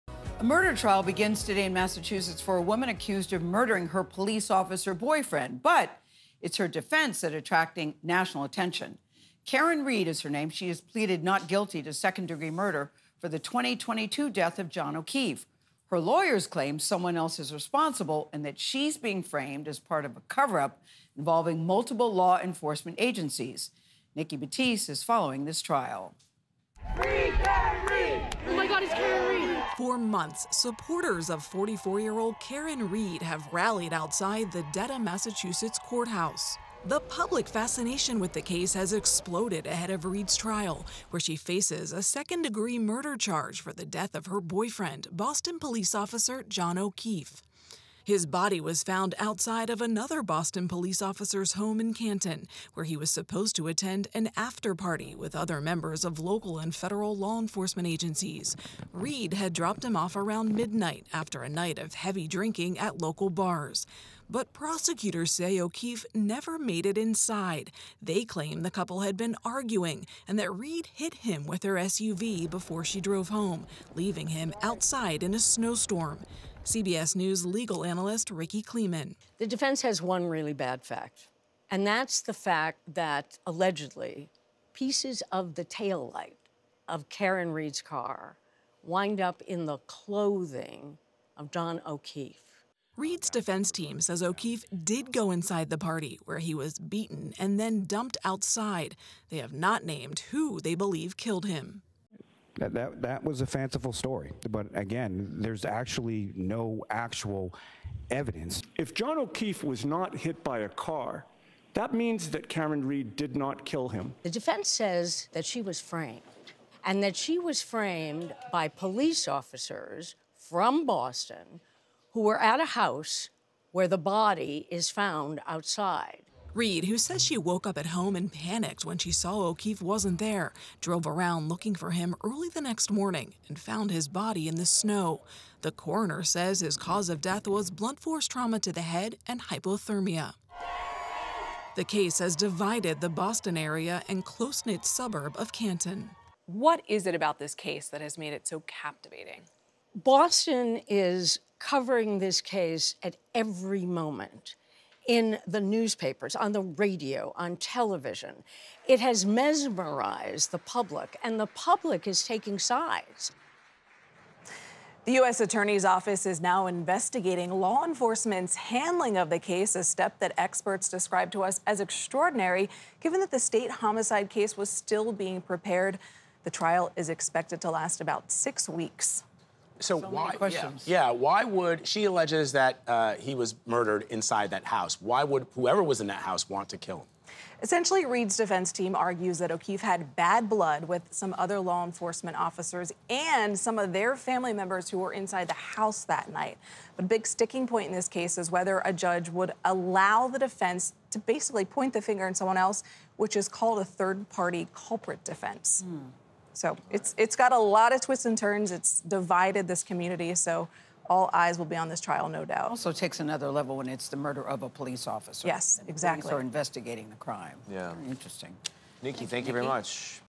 Karen Read murder trial begins, woman accused of killing her police officer boyfriend
A murder trial starts Tuesday for Karen Read, a Massachusetts woman accused of murdering her boyfriend, who was a police officer, in 2022. Her lawyers say Read is being framed as part of a cover up involving multiple law enforcement agencies, and that someone else is responsible.